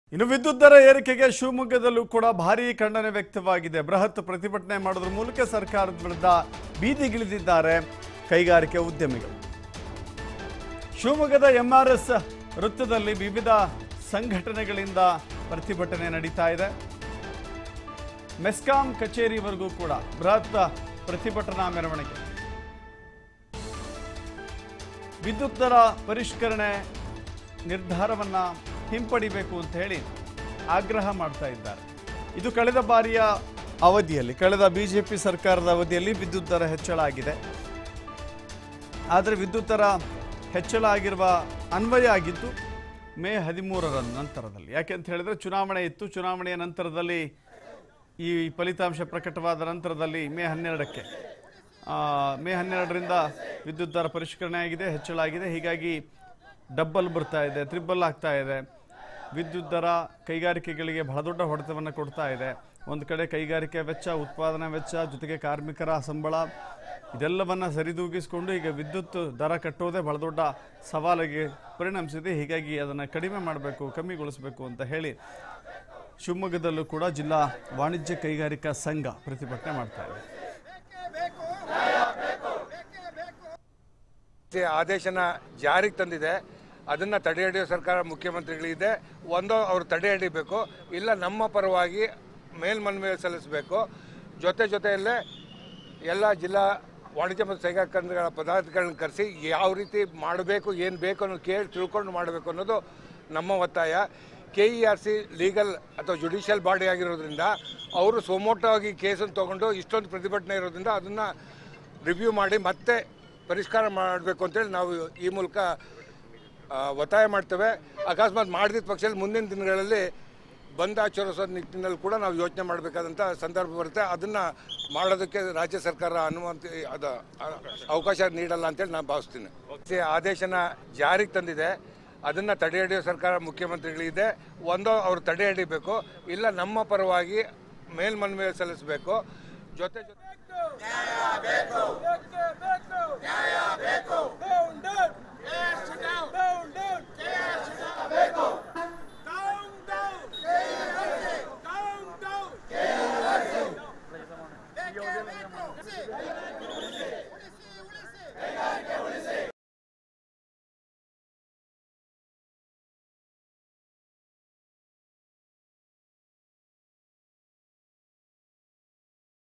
Electricity Price Hike:ವಿದ್ಯುತ್ ದರ ಏರಿಕೆಗೆ ಶಿವಮೊಗ್ಗದಲ್ಲಿ ಬೃಹತ್ ಪ್ರತಿಭಟನೆ! | Shivamogga | TV5 Kannada
Electricity Price Hike :ವಿದ್ಯುತ್ ದರ ಏರಿಕೆಗೆ ಶಿವಮೊಗ್ಗದಲ್ಲಿ ಬೃಹತ್ ಪ್ರತಿಭಟನೆ! | Shivamogga Protest | TV5 Kannada